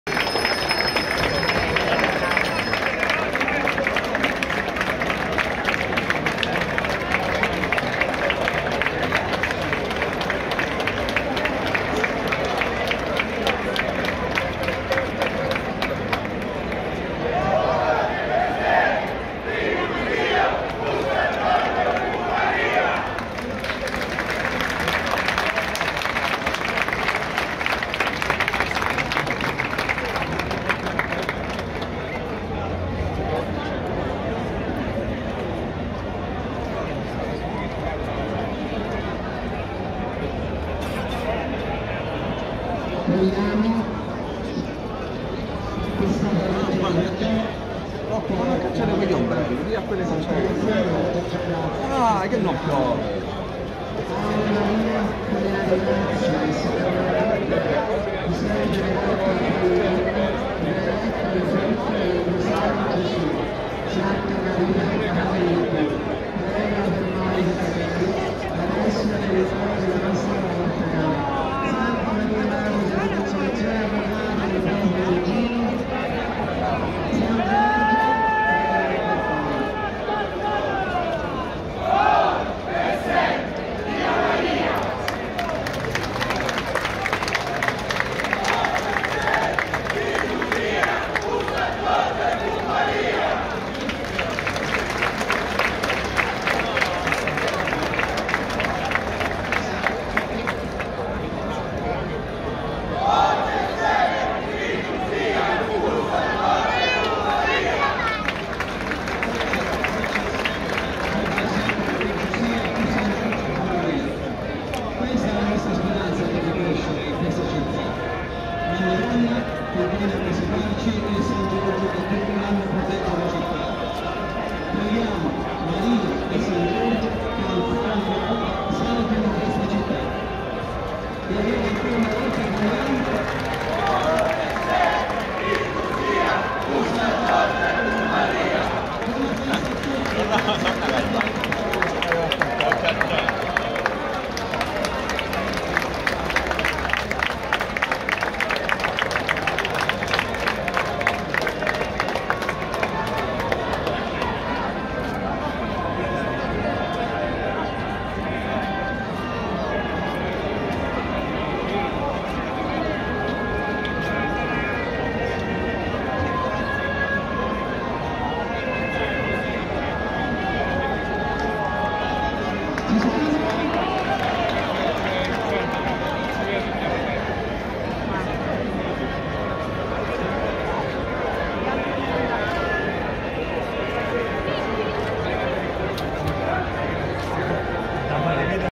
Reggio Calabria, primo storico incontro tra la Madonna e San Giorgio sul corso Garibaldi
Per la prima volta nella storia, su volontà di don Nuccio Cannizzaro, durante la processione di questa mattina la Madonna della Consolazione si è fermata per salutare San Giorgio di fronte la chiesa di San Giorgio al Corso. Il Santo Patrono della città incontra e accoglie la Madonna a tutela dell'identità comune di Reggio Calabria. Tutti i dettagli su